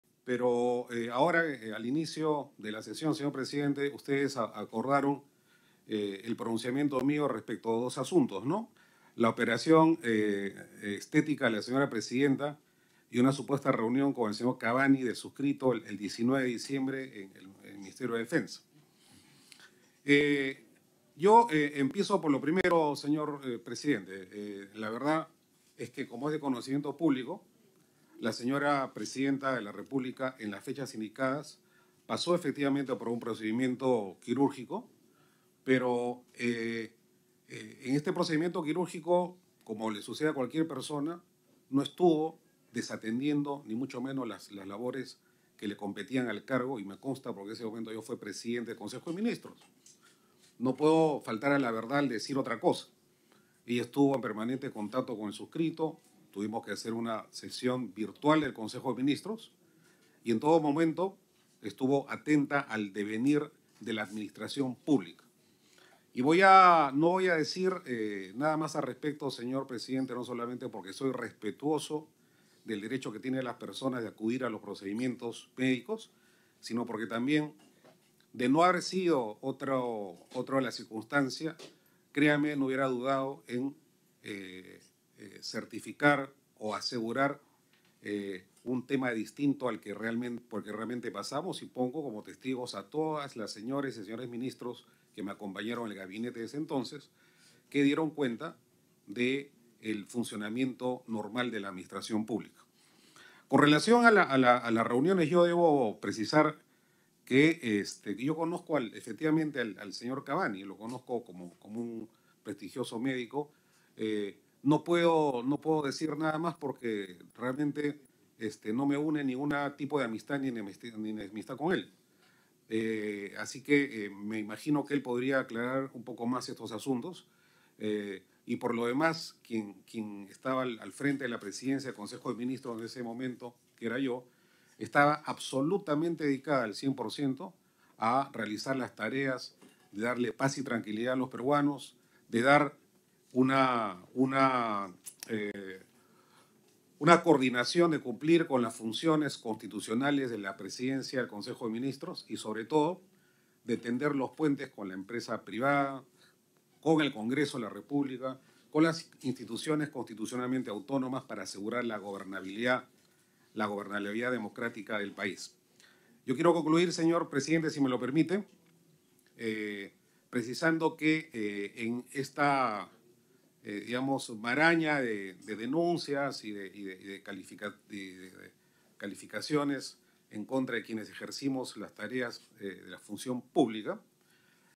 Alberto Otárola confirma operación de Dina Boluarte: "Tuvimos que hacer una sesión virtual"
El ex primer ministro, Alberto Otárola, acudió a la Comisión de Fiscalización del Congreso de la República y, entre varios otros temas más, habló sobre el procedimiento quirúrgico de la presidenta del Perú, Dina Boluarte. Estos son los detalles que dijo.